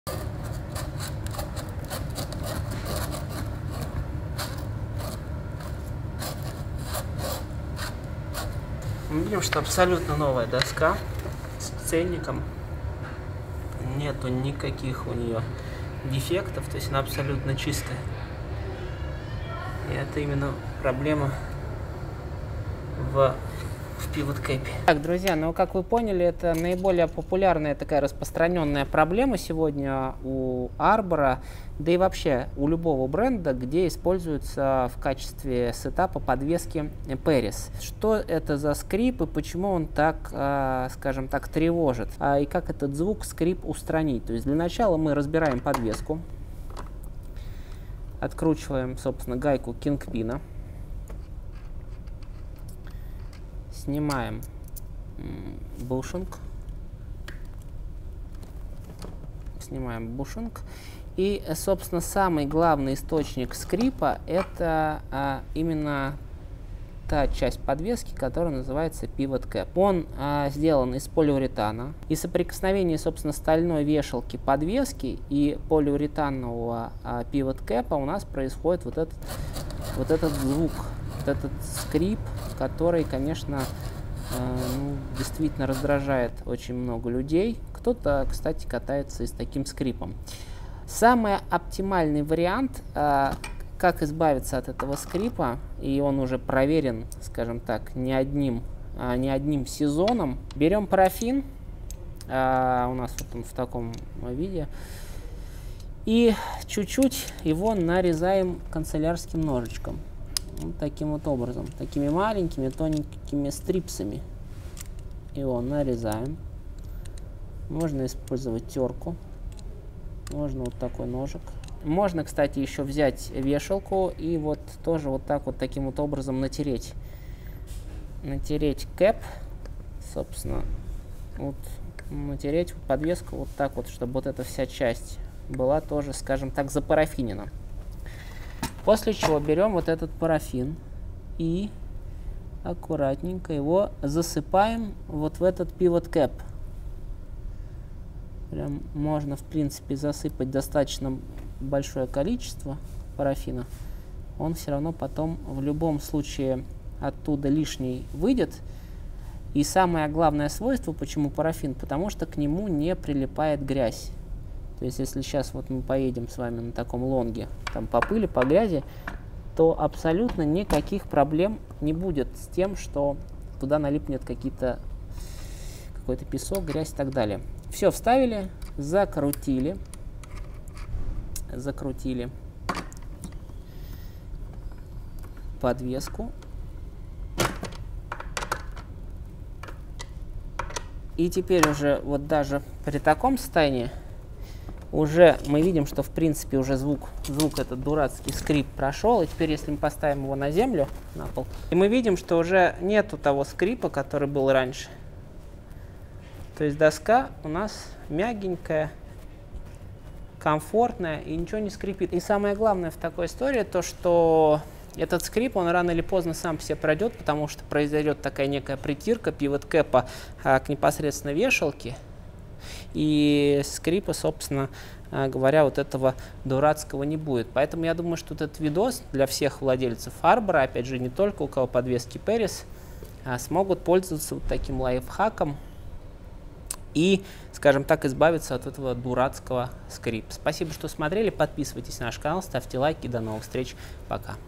Скрипит подвеска на лонгборде. Что делать?
Частая проблема - скрип подвески на новом лонгборде. Что это и как от него избавиться?
Неприятный звук образуется от соприкосновения стальной вешалки подвески и полиуретанового пивот кэпа. Чтобы избавиться от него - разберите подвеску и поместите в пивот кэп стружку из парафина.

В видео мы использовали парафин для сноуборда!

📖 Читайте статью в нашем блоге 👉🏽 "Как выбрать лонгборд: гид для новичков. Часть #1" 👉🏽